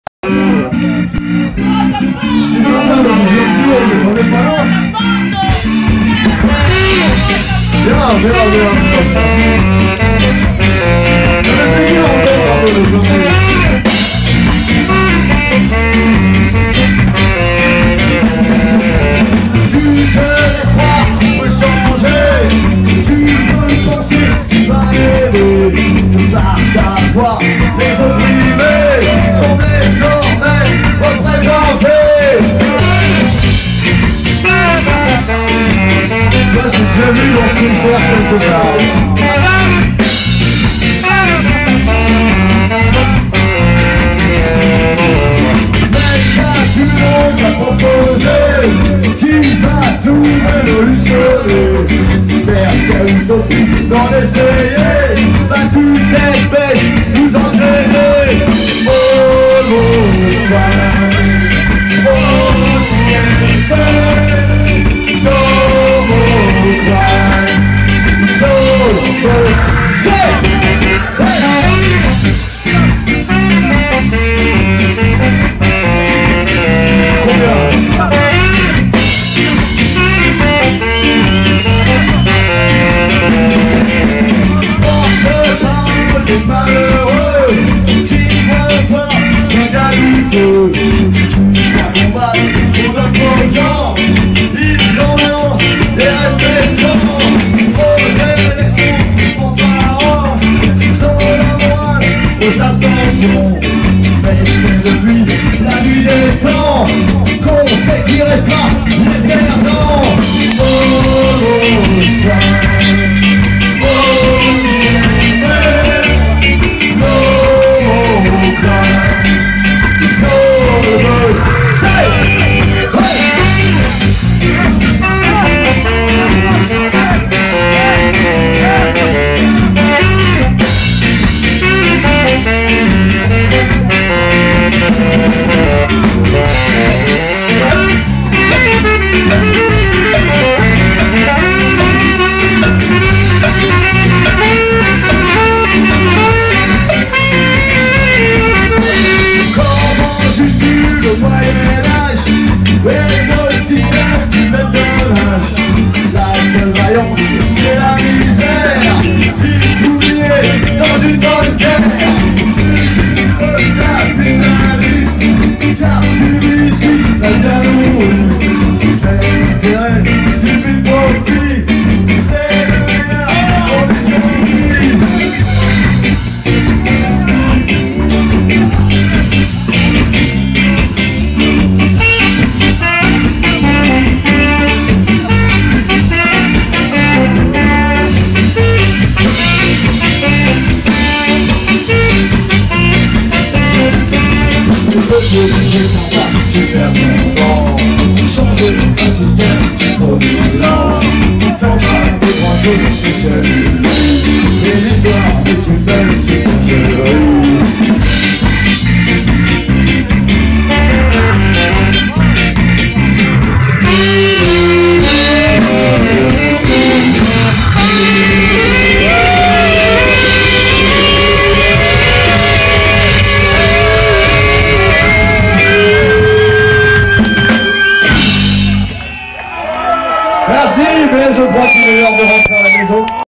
Skarface 3 juillet 2009 preignan (32)
Skarface au festival rock'n'stock a preignan le 3 juillet 2009